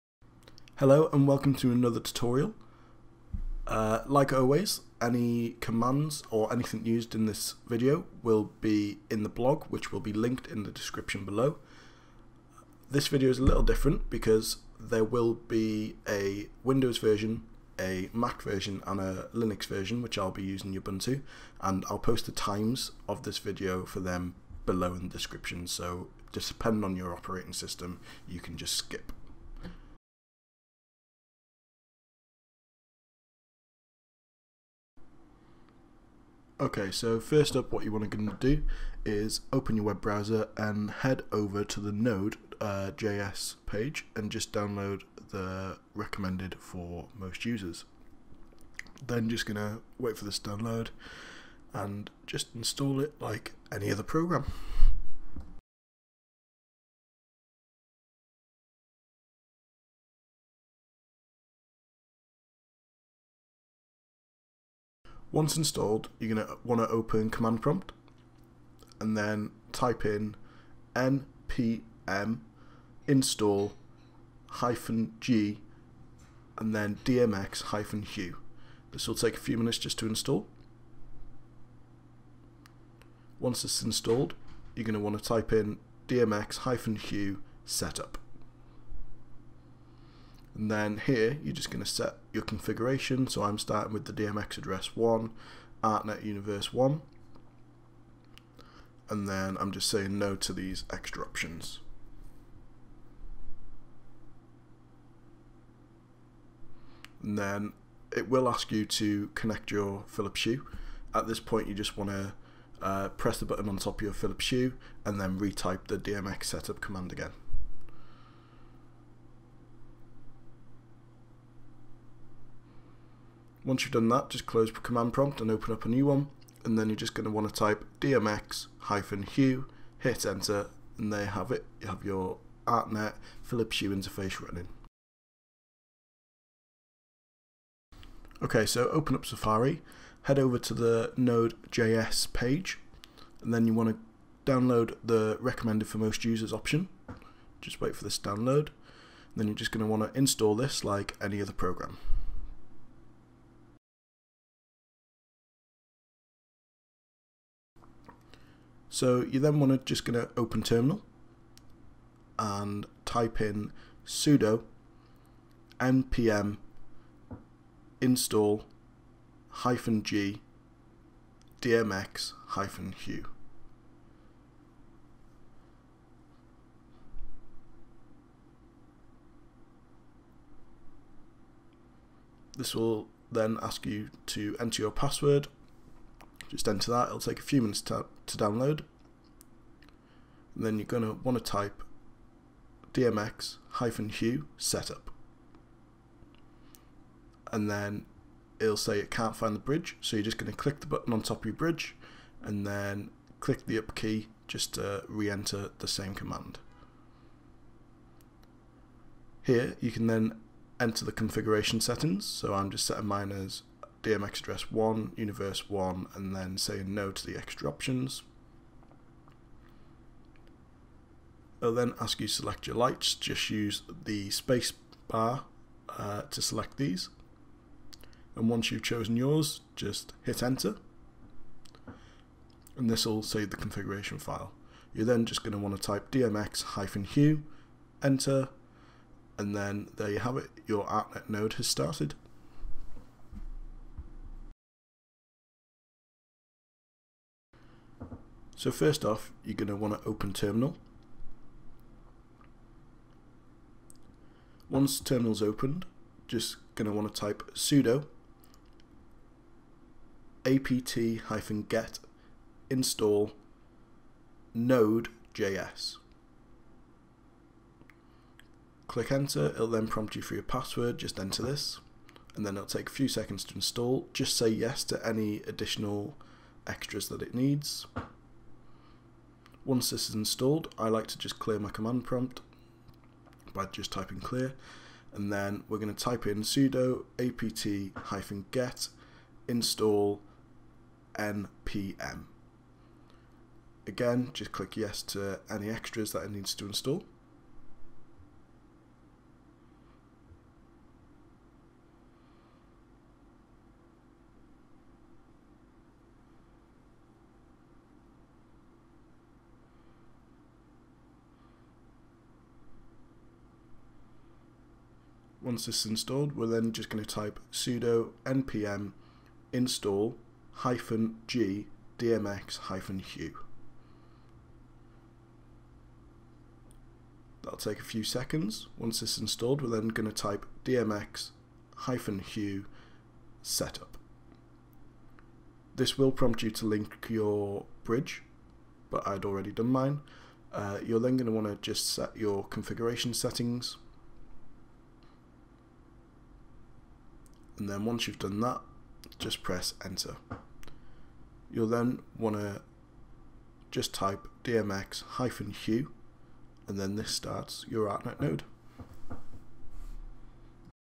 Control your Phillips Hue Via DMX!!! Windows, Mac and Linux
Have you ever wanted to be able to control Phillips hue during a show, well now you can.